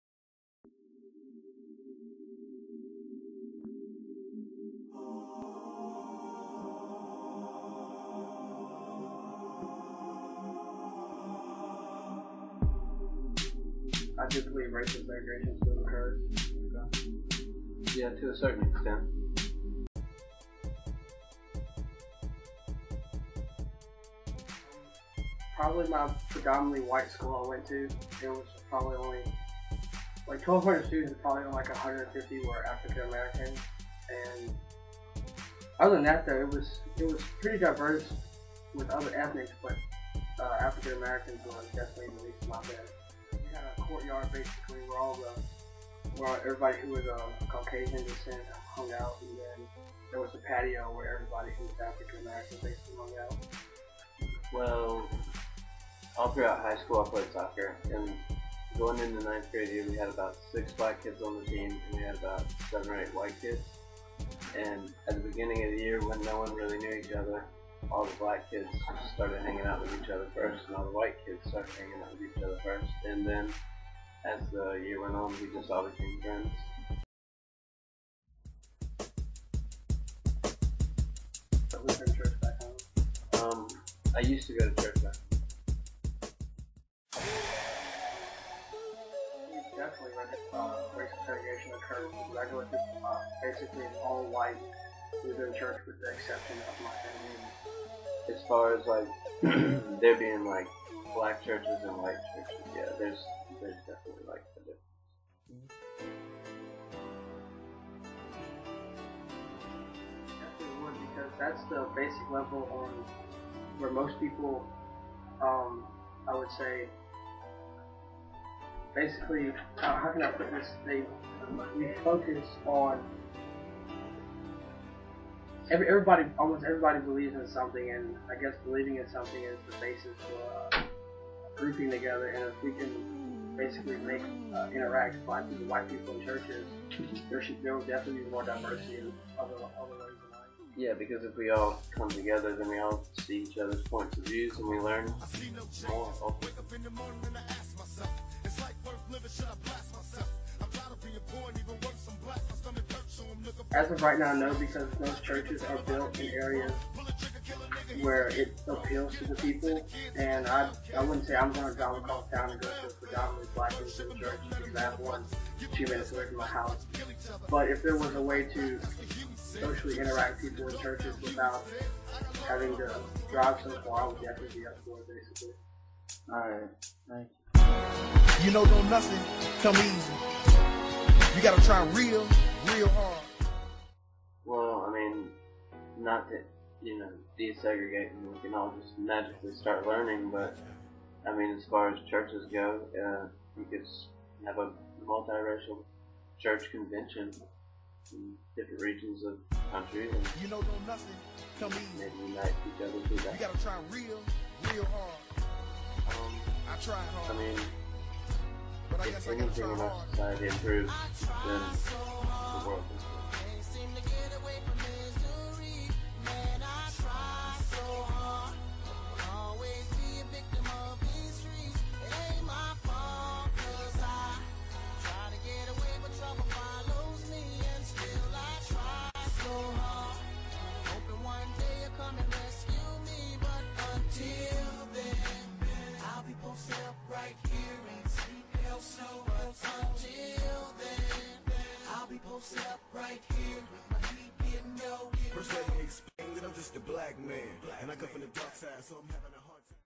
Modern Day Segregation in America's Churches
Students in the Introduction to African-American Studies class at the College of Charleston were asked to complete an "activism project" in which they tried to address a racial issue that they identified in their community.